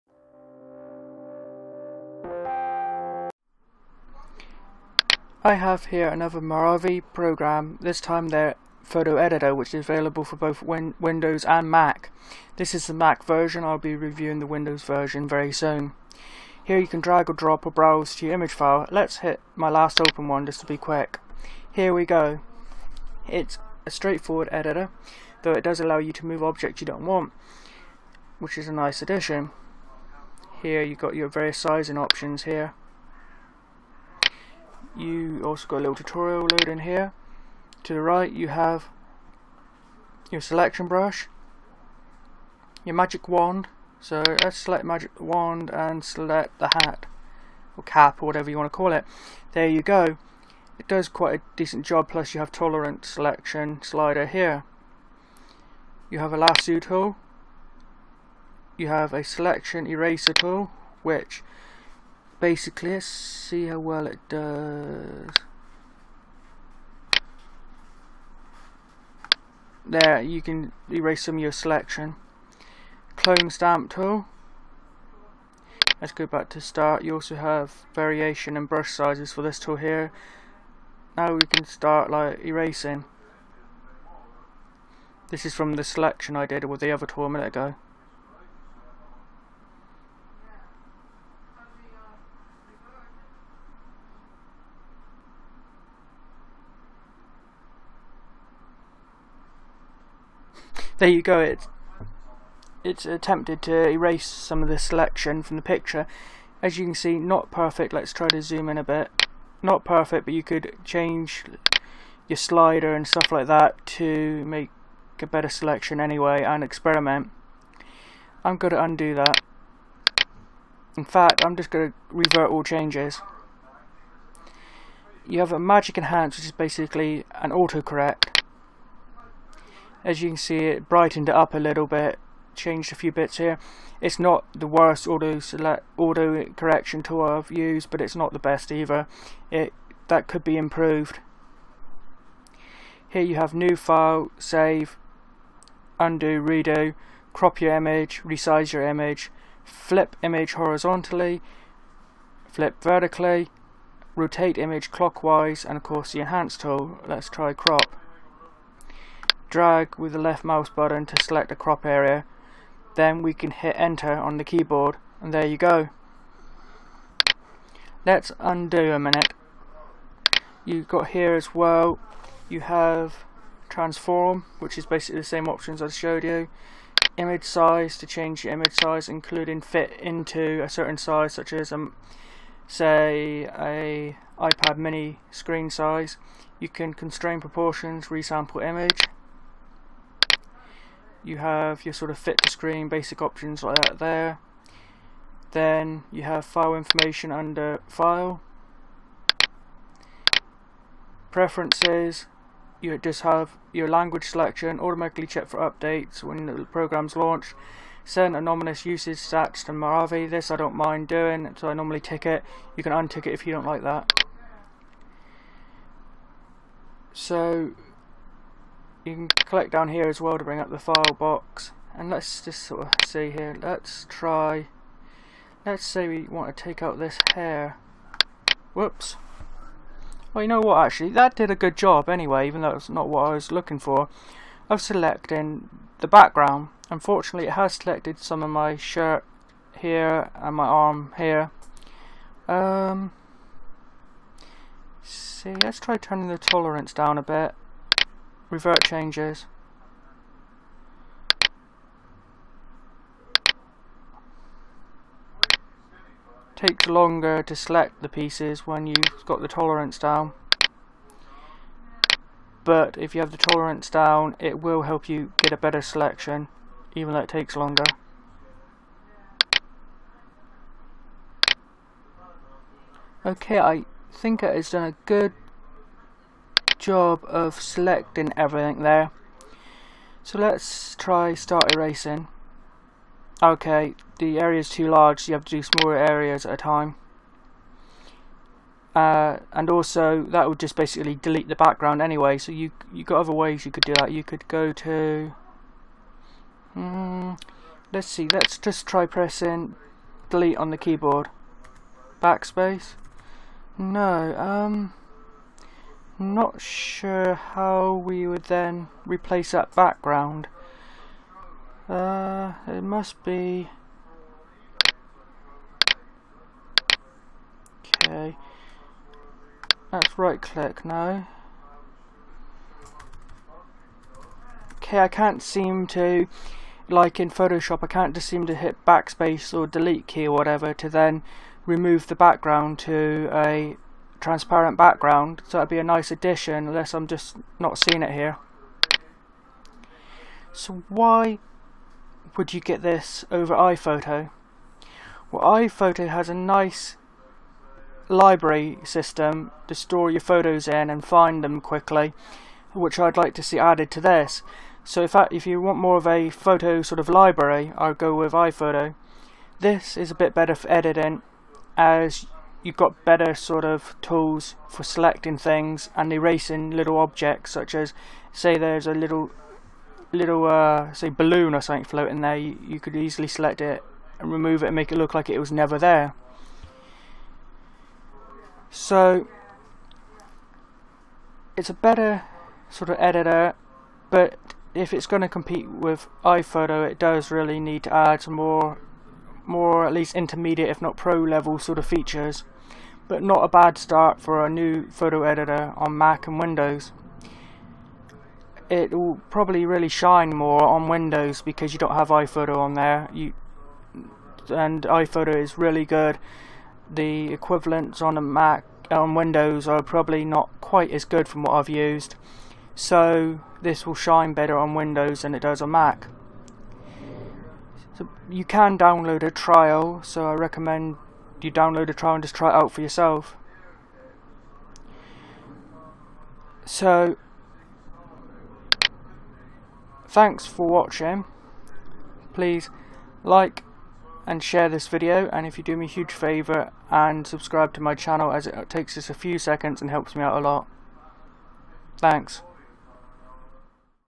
Movavi Photo Editor for Mac: Review
My review for the basic Photo Editor by Movavi, that is particularly good at removing unwanted objects from photos.

Price: £15.67 (with VAT)

Version Reviewed: 1.1.0

Thanks to Movavi for making this review possible.